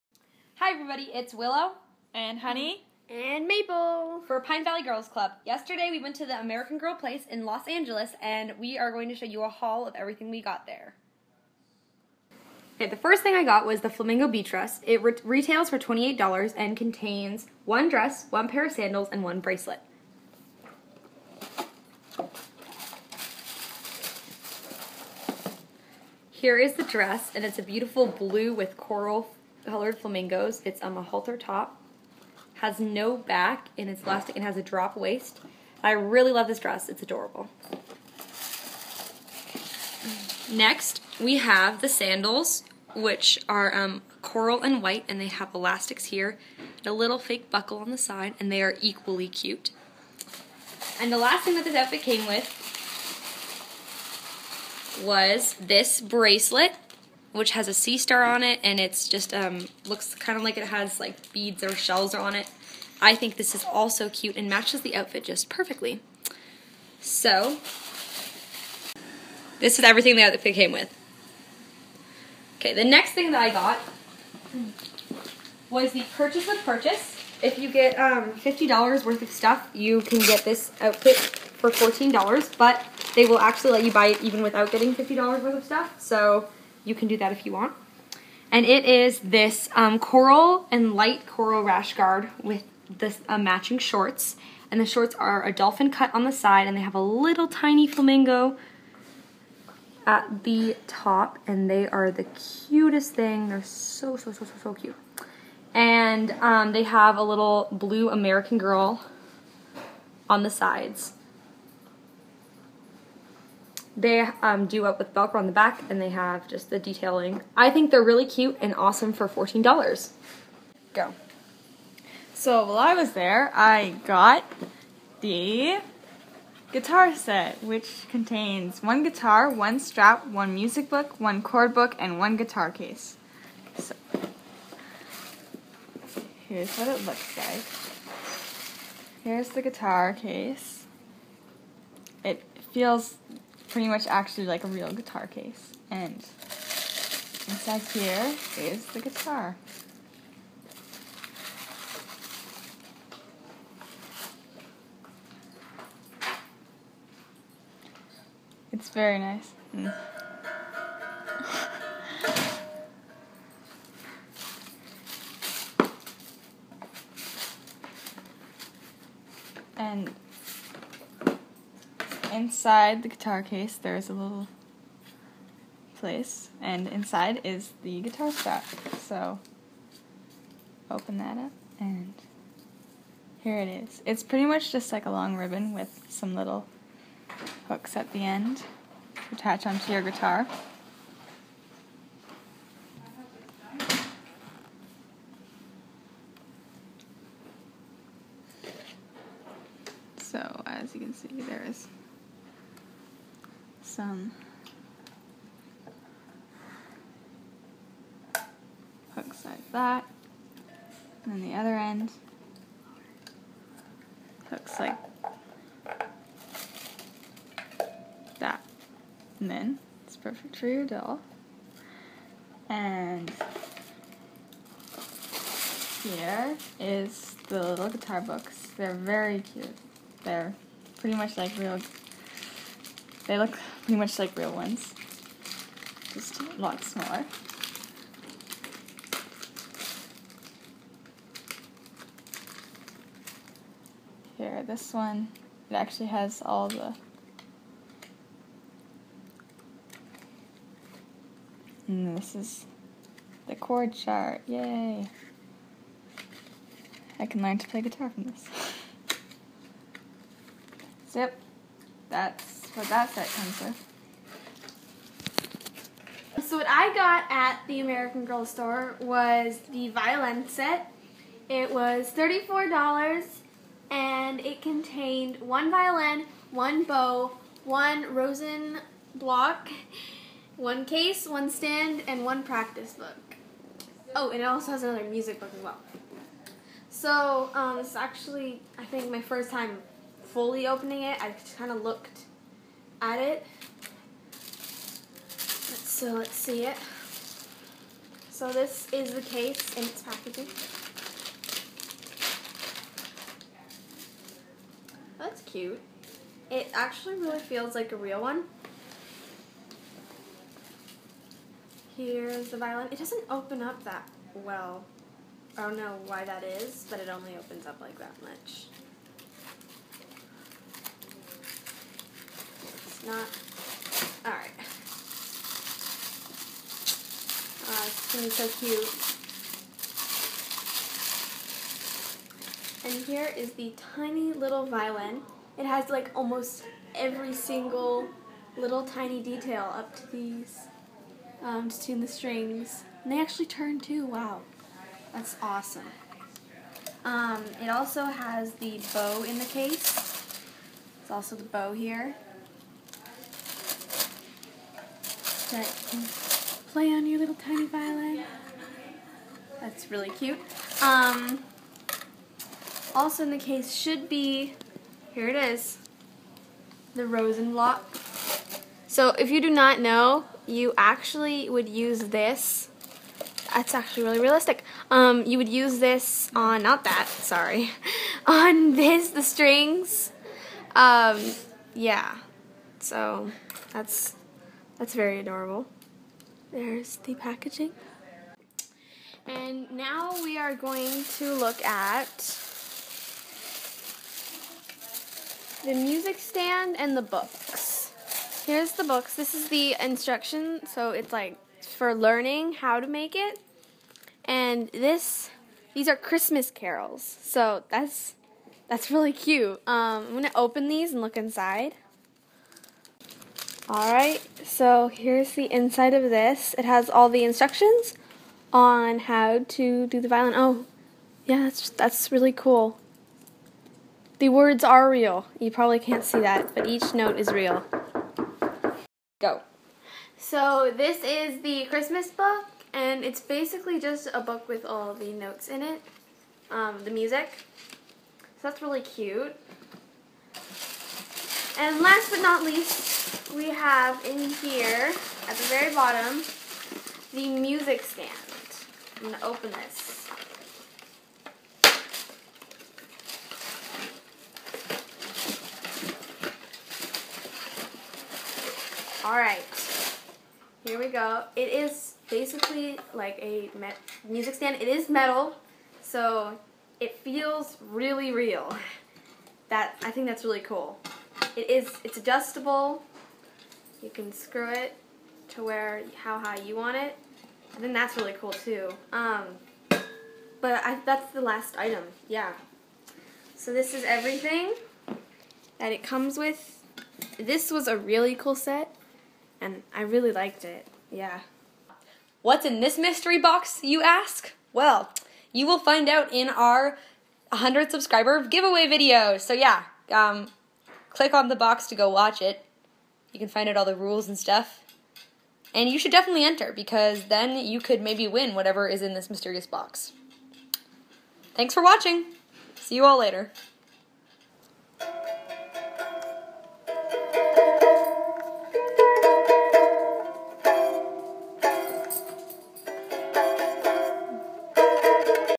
American Girl Place LA Haul 2015
Hi everyone! We went to the American Girl Place in Los Angeles and bought some stuff, so here is a video showing it all to you! Sorry it is so long... We totally recommend all of the items we bought. They are fabulous!